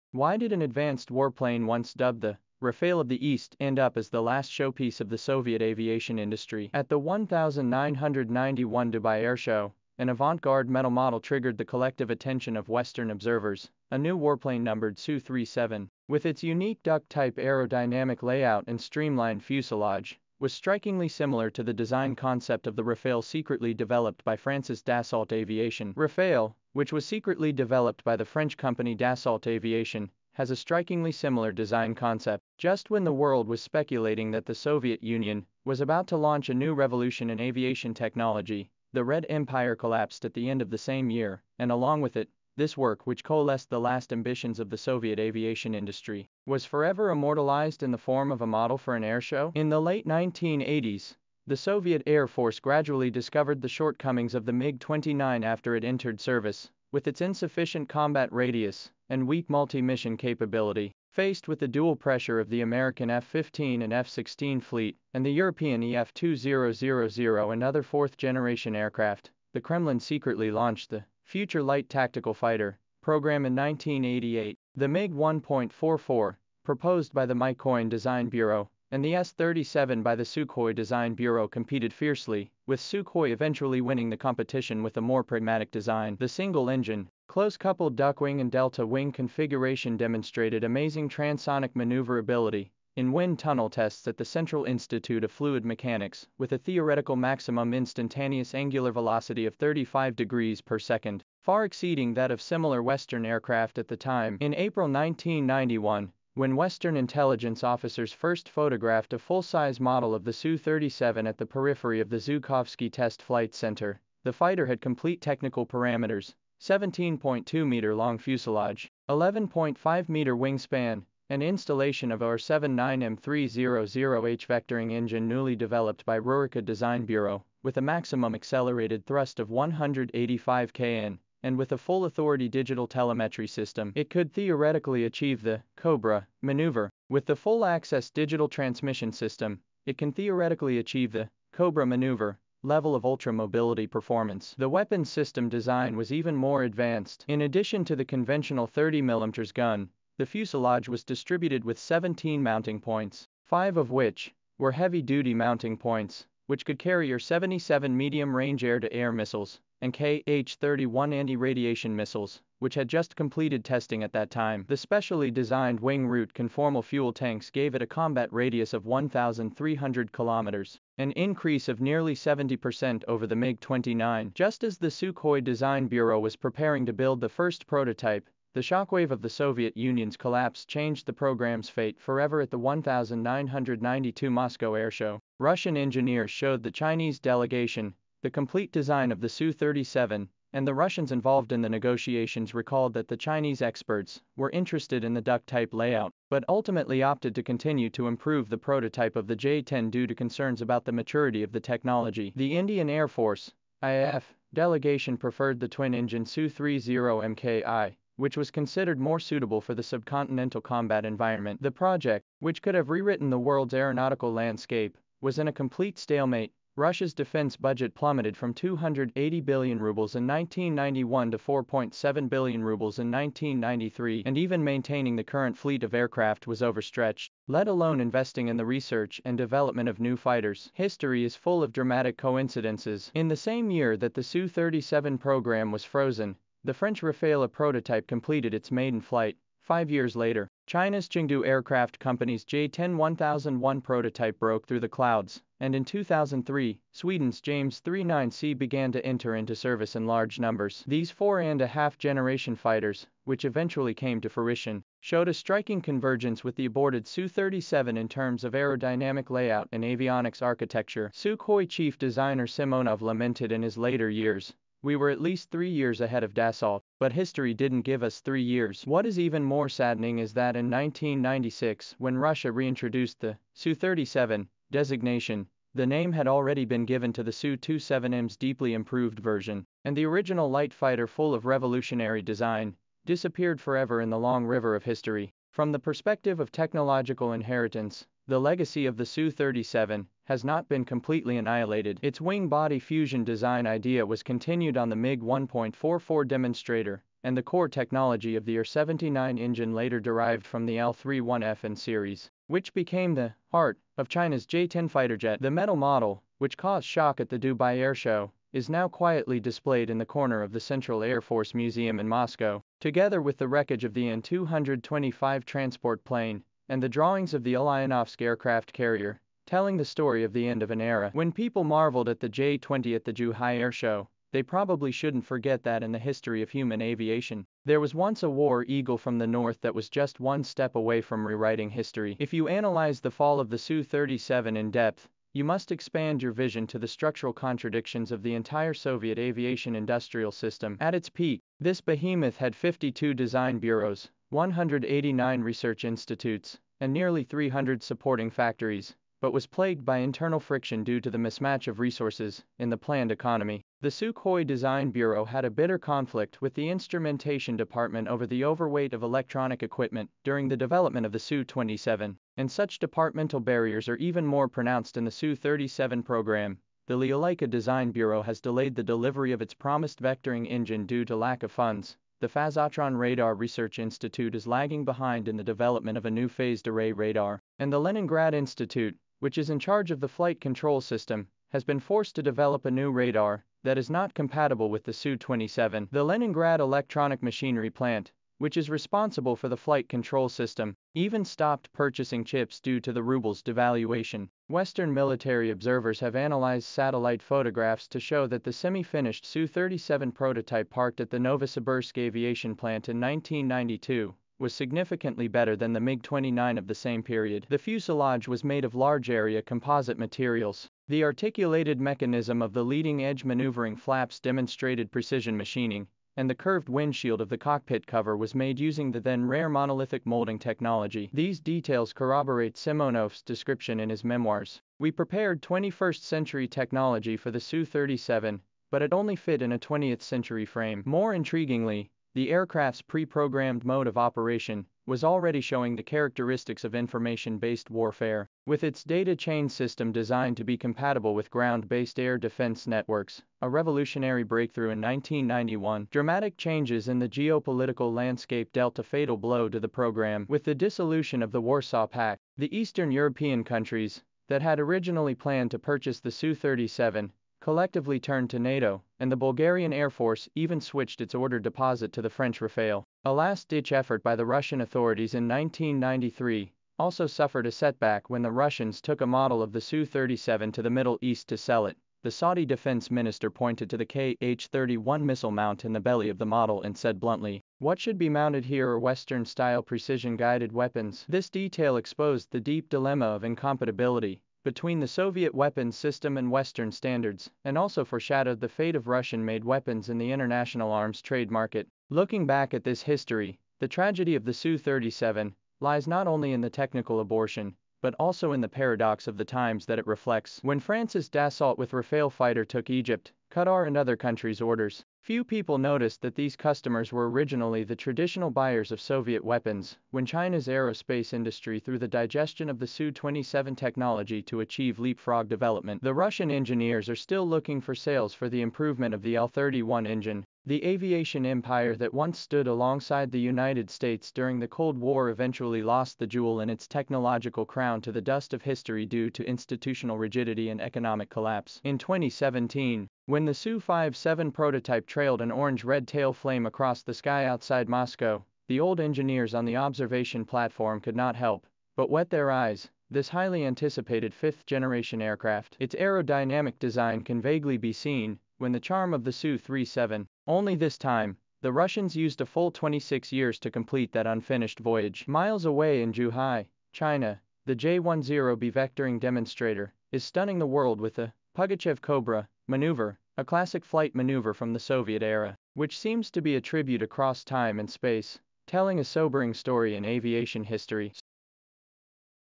Why was only one Su37 fighter built, the oldest predecessor of the J-10?
Why did an advanced warplane once dubbed the “Rafale of the East” end up as the last showpiece of the Soviet aviation industry? At the 1991 Dubai Airshow, an avant-garde metal model triggered the collective attention of Western observers - a new warplane numbered Su37, with its unique duck-type aerodynamic layout and streamlined fuselage, was strikingly similar to the design concept of the Rafale secretly developed by France's Dassault Aviation. Rafale, which was secretly developed by the French company Dassault Aviation, has a strikingly similar design concept. Just when the world was speculating that the Soviet Union was about to launch a new revolution in aviation technology, the Red Empire collapsed at the end of the same year, and along with it, this work, which coalesced the last ambitions of the Soviet aviation industry, was forever immortalized in the form of a model for an airshow.